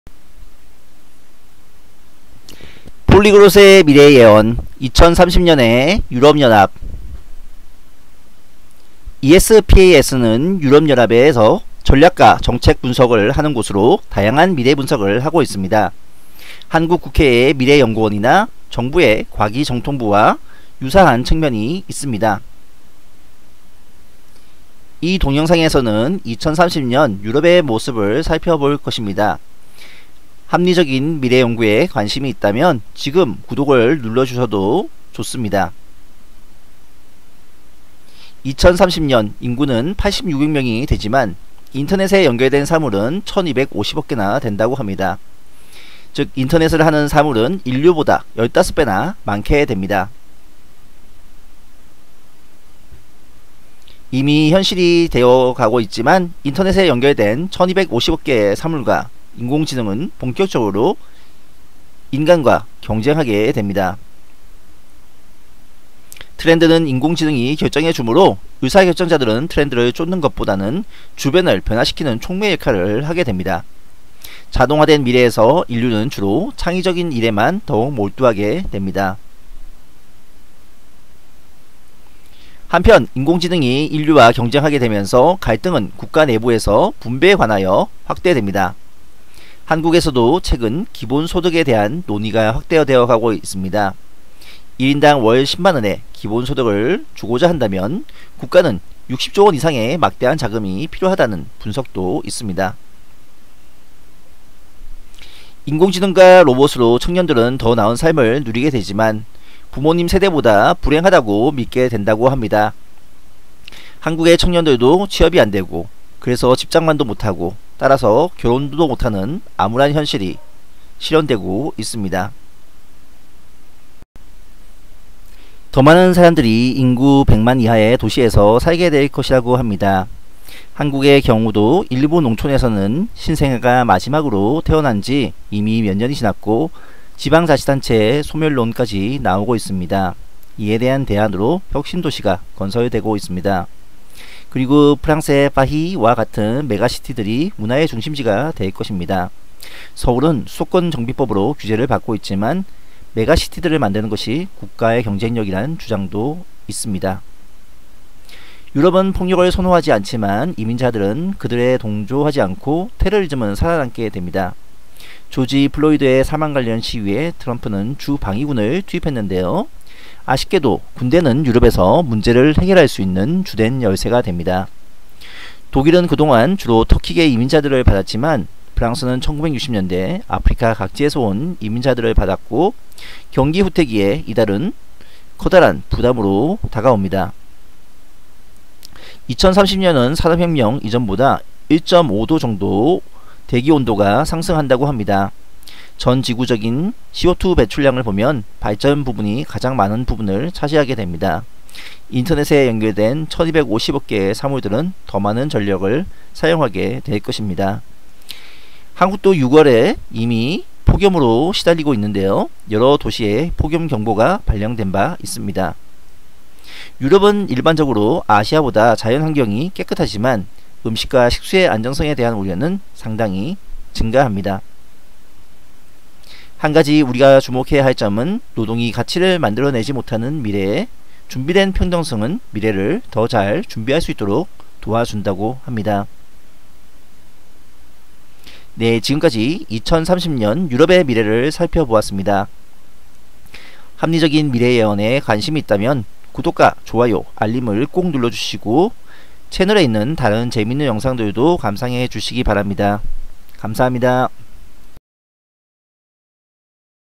[정책연구4만건] 2030년 유럽의 충격적 미래, 1250억개의 사물들이 인터넷에 연결,인공지능은 인류와 경쟁, 메가시티가 중요해짐 테러리즘 군대가 문제해결 준비된 평등성이 해결책
ESPAS는 유럽연합에서 전략과 정책을 분석하는 곳으로 다양한 미래분석을 하고 있습니다. 한국국회의 미래연구원이나 정부의 과기정통부와 유사한 측면이 있습니다 이 동영상에서는 2030년 유럽의 모습을 살펴볼 것입니다. 합리적인 미래연구에 관심이 있다면 지금 구독을 눌러주셔도 좋습니다

2030년 인구는 86억이 되지만 인터넷에
연결된 사물은 1250억개나 된다고 합니다.
인터넷을 하는 사물은 인류보다
15배나 많게 됩니다.
이미 현실이 되고 있지만 인터넷에 연결된
1250억개의 사물과 인공 지능은 본격적으로
인간과 경쟁하게 됩니다.
트렌드는 인공지능이 결정해 주므로
의사결정자들은 트렌드를 쫓는 것보다 주변을
변화시키는 촉매역할을 하게 됩니다.
자동화된 미래에서 인류는 주로
창의적인 일에만 더욱 몰두하게 됩니다.
한편 인공지능이 인류와 경쟁하게 되면서
갈등은 국가내부에서 분배에 관하여 확대됩니다.
한국에서도 최근 기본소득에 대한 논의가
확대되고 있습니다. 1인당 월10만원의 기본소득을
주기위해 국가는 60조원 이상의 막대한 자금이
필요하다고 합니다.
인공지능과 로봇으로 청년들은 더 나은 삶을 누리지만
부모님세대보다 불행하다고 믿게된다고 합니다.
한국의 청년도 취업이 안되고, 그래서 집장만도
못하고 따라서 결혼도 못하는 암울한 현실이
더 많은 사람들이 인구100만이하의 도시에서
살게될 것이라고 합니다. 한국의 경우
일부 농촌에서는 신생아가 마지막으로
태어난지 몇년이 지났고, 자치단체 소멸론이 나오고
혁신도시가 건설되고 있습니다.
그리고, 빠히와 같은 메가시티들이 문화의 중심지가
될 것이라고 전망합니다. .
서울은 수도권정비법으로 규제를 받고 있지만
메가시티들이 국가의 경쟁력이란 주장도 있습니다
유럽은 폭력을 선호하지 않지만, 이민자들은
그들에 동조하지 않고 테러리즘은 살아남습니다.
조지 플로이드의 사망관련 시위에 트럼프는
주방위군을 투입했는데요 아쉽게도
군대는 유럽에서 문제를 해결할 수 있는 주된
열쇠가 됩니다.
독일은 주로 터키계 이민자를 받았지만
프랑스는1960년대 아프리카 각지에서 온 이민자들을
받아 경기후퇴기에 이들은 커다란 부담으로
다가옵니다.
2030년은 산업혁명이전보다 1.5도
대기온도가 상승한다고 합니다.
전지구적인 CO2배출량을 보면
발전부분이 가장 많은 부분을 차지하게 됩니다
 인터넷에 연결된 1250억개의
사물들은 더 많은 전력을 사용하게 될 것입니다.
한국도 6월에 이미 폭염으로 시달리고 있으며
여러 도시에 폭염경보가 발생되었습니다.
유럽은 아시아보다 자연환경이 깨끗하지만
음식과 식수의 안정성에 대한 우려는 상당히
증가합니다.
한가지 우리가 주목해야할 점은
노동이 가치를 만들지 못하는 미래 준비된 평등성은
미래를 더 잘 준비할 수 있도록 도와준다고 합니다.
지금까지 2030년 유럽의 미래를 살펴보았습
니다. 합리적인 미래예언에 관심이 있다면
채널의 다른 영상도 감상해 주시기 바랍니다